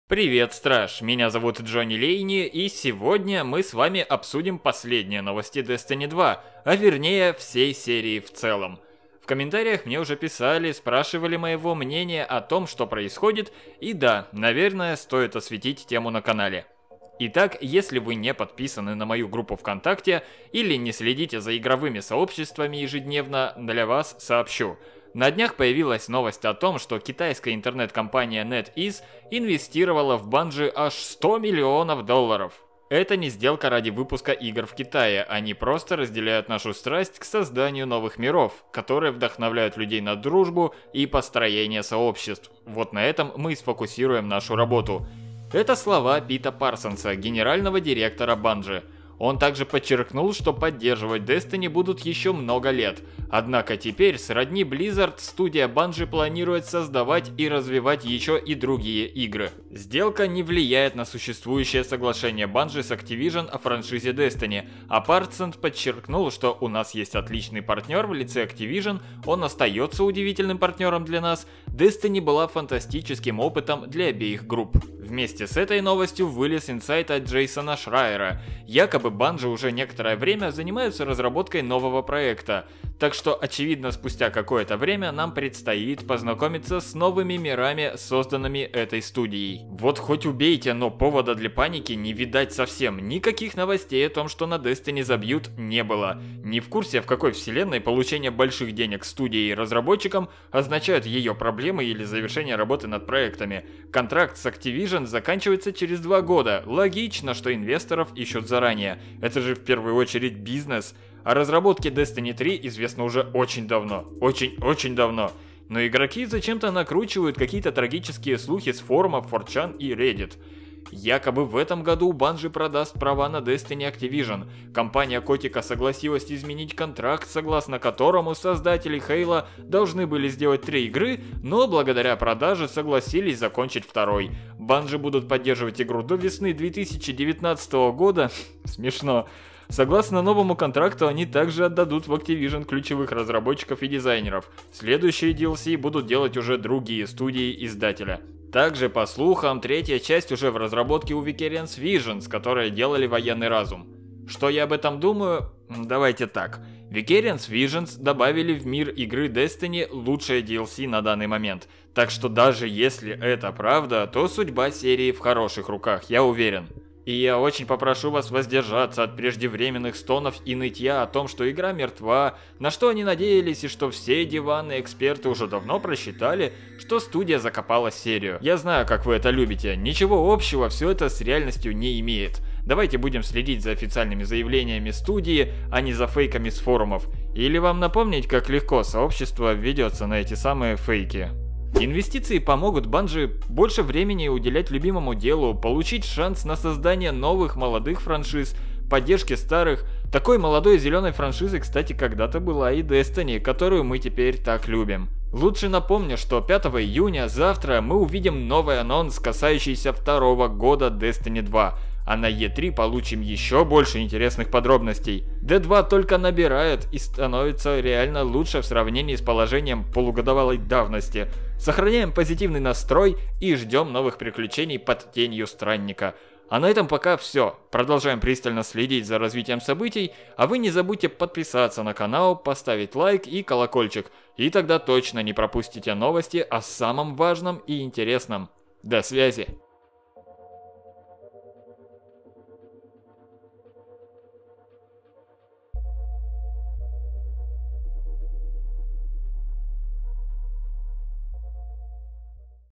Destiny 2. Разработка новой игры от Bungie. Судьба серии Destiny.
Очень много слухов появилось вокруг недавней новости об инвестициях в Bungie со стороны китайской компании. Рассказываю почему ничего страшного не произошло и почему нельзя верить всему подряд.